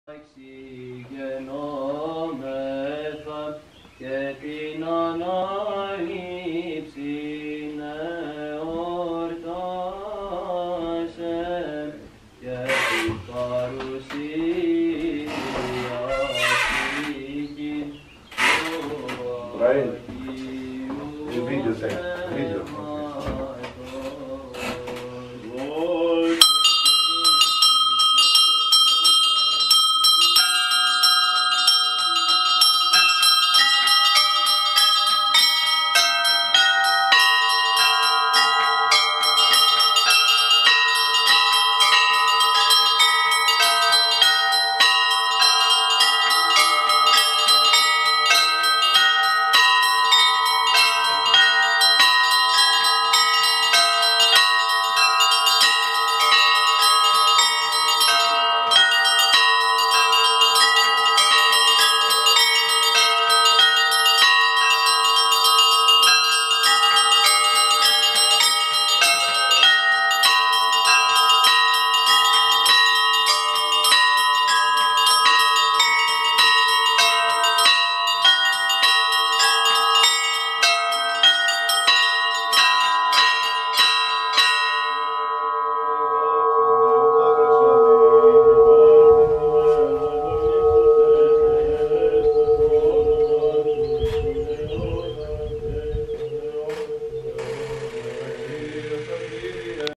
Sunrise/ Doxology Bells on Mt Athos
Elder Antipas of Aghia Anna on Holy Mountain Athos plays the bells to start the Doxology